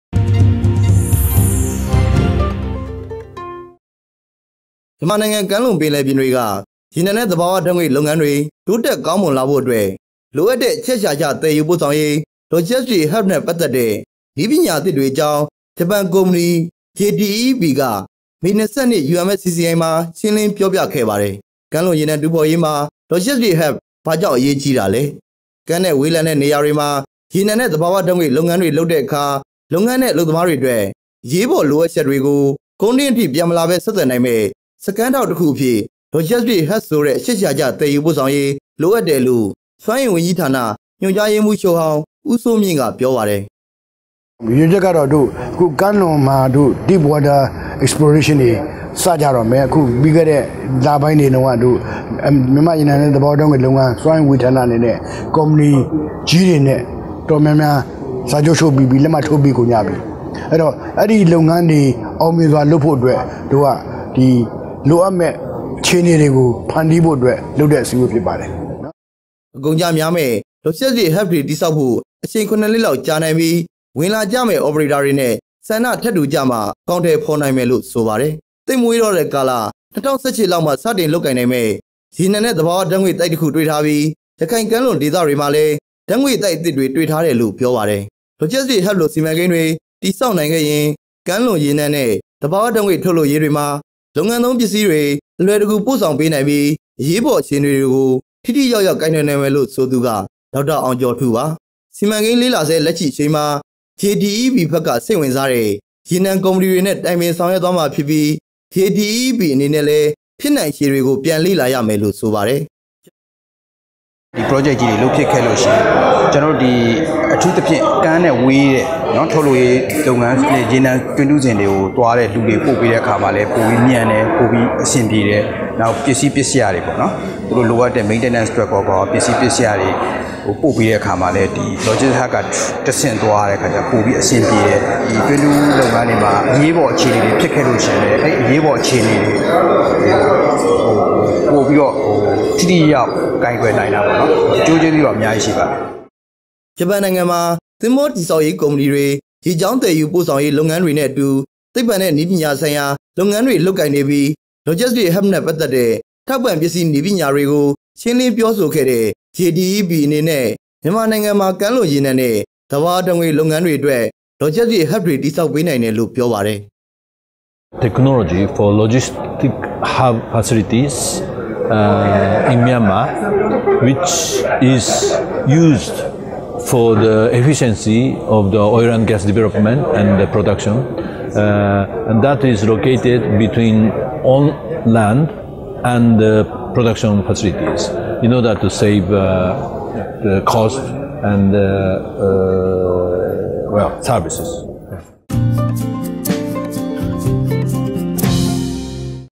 က​မ​္း​လြ​န​္ေ​ရ​နံ​တူး​ေ​ဖာ​္ေ​ရး Logistic Hub ေ​တြ ဘယ​္ေ​တာ့​ေ​ဆာ​က​္ႏိုင္မ​လဲ
ျ​မန္မာႏ​ိုင္ငံ က​မ​္း​လြန္ပင္လ​ယ​္ျ​ပ​င​္ေ​တြ​က ေ​ရ​နံ​နဲ​႔​သ​ဘာ​ဝ​ဓာ​တ​္ေ​ငြ​႔​လုပ္င​န​္းေ​တြ တိုး​တ​က​္ေ​ကာ​င​္း​မြန္လာ​ဖို​႔​အတြ​က​္ လို​အပ္တဲ့ အခ်က္အခ်ာက် သယ္ယူ​ပို​႔ေ​ဆာ​င​္ေ​ရး Logistic Hub နဲ​႔​ပတ္သတ္တဲ့ န​ည​္း​ပညာ​အသ​စ​္ေ​တြ​အေ​ၾ​ကာ​င​္း ဂ်​ပန္ကု​မ​ၸ​ဏီ J-DeEP က ေ​မ ၂​၂ UMFCCI မွာ ႐ွ​င​္း​လ​င​္းေျ​ပာ​ျ​ပ​ခဲ့​ပါ​တ​ယ​္။